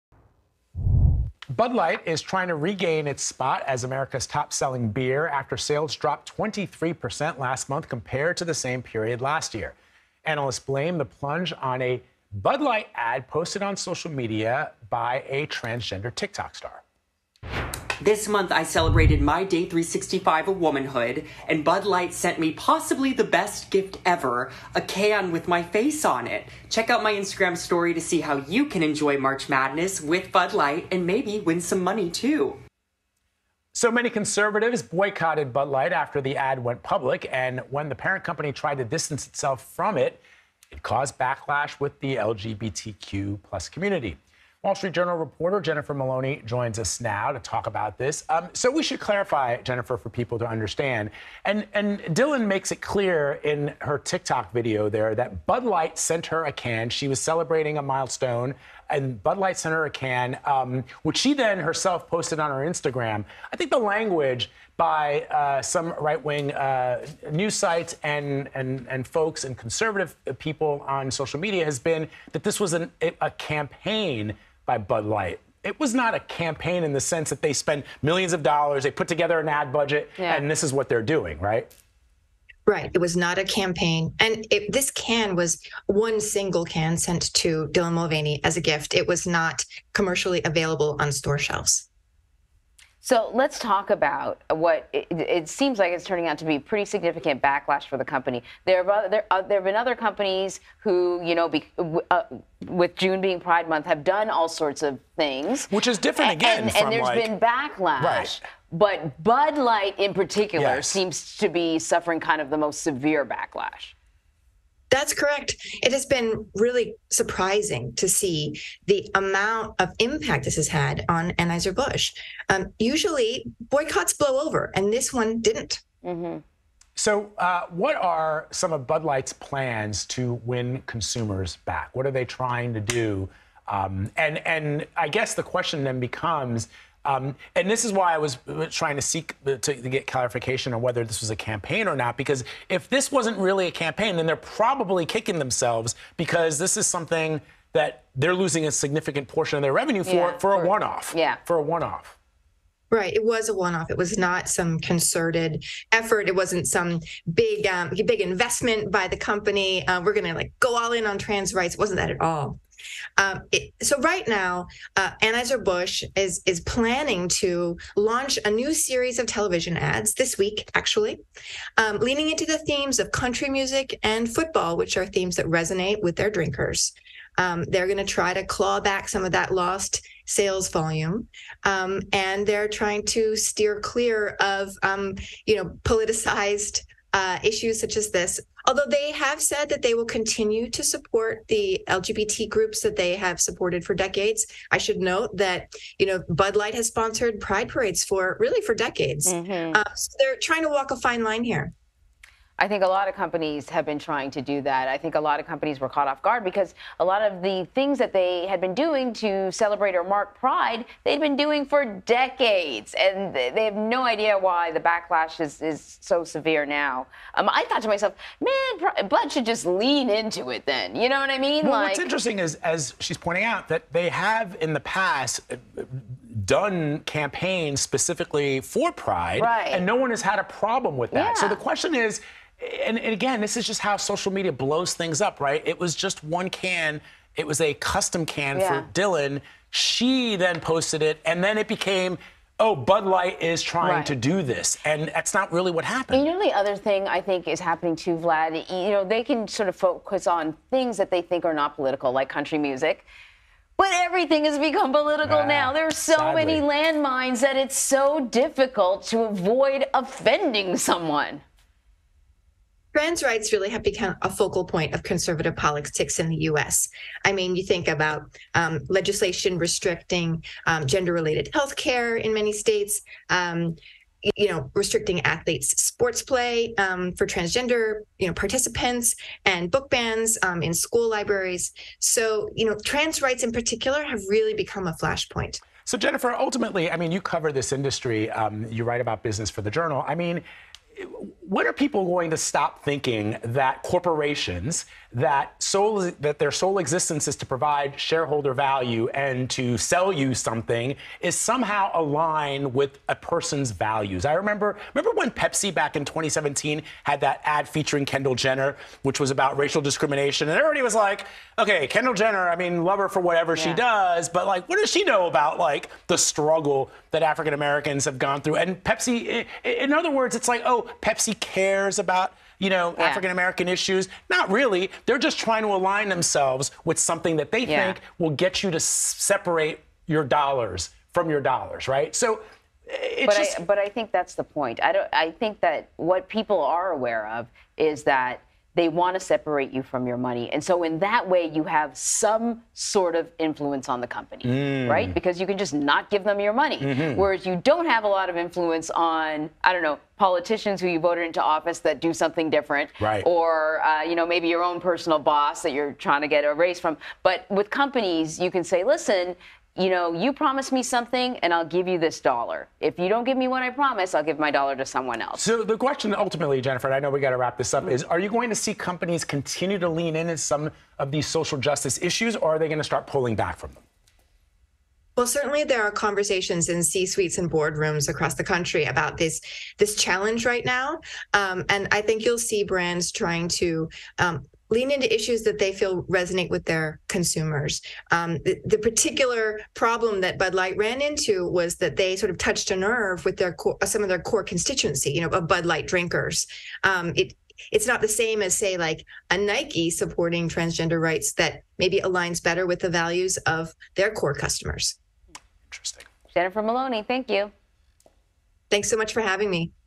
Bud Light and Dylan Mulvaney: Why the extreme backlash from a single social media post?
Bud Light is trying to regain its spot as America's top-selling beer after sales dropped 23% last month compared to the same period last year. Analysts blame the plunge on backlash against a social media post by transgender TikTok star Dylan Mulvaney. Wall Street Journal reporter Jennifer Maloney joins CBS News with more.